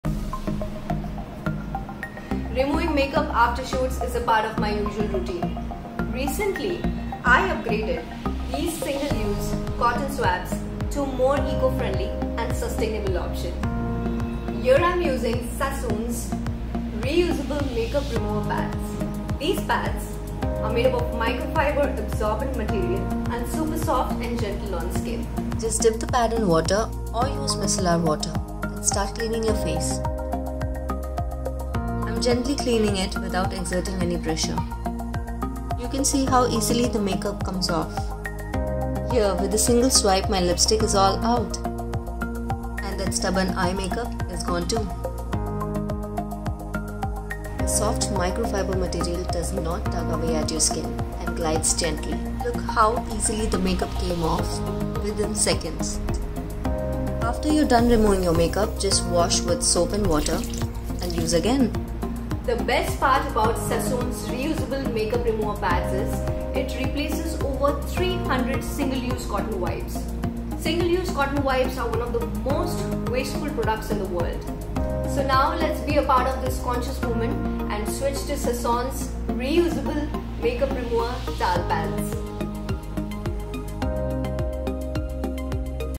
Wonderful reusable makeup remover cotton pads || Neha Janpandit
As removing makeup after shoots is a part of my daily usual routine so recently I have upgraded these single-use cotton swabs to a more eco-friendly and sustainable option.

Thank You @sassoonfab for bringing such wonderful reusable makeup remover cotton pads

Upgrade your makeup routine with Sassoon's gentle and eco-friendly reusable makeup remover pads. Remove makeup in seconds without harming your skin and join the conscious movement by ditching single-use wipes.